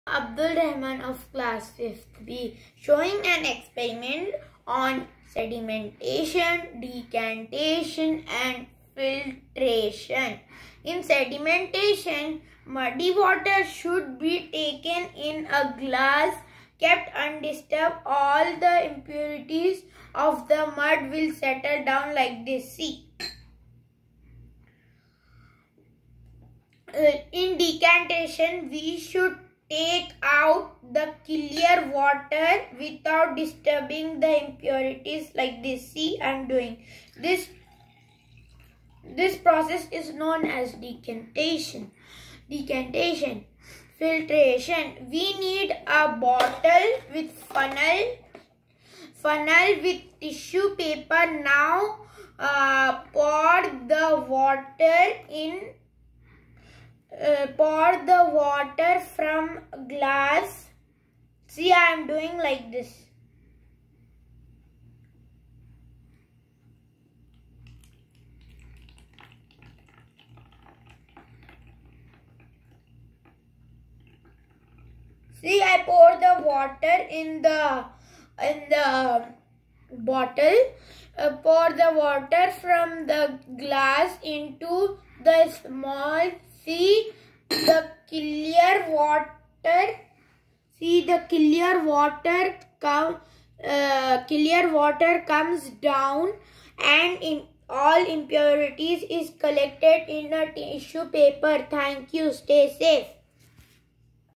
REMOVAL OF INSOLUBLE IMPURITIES FROM WATER
AN ACTIVITY BY CLASS V STUDENT
SEDIMENTATION ,  DECANTATION  AND FILTRATION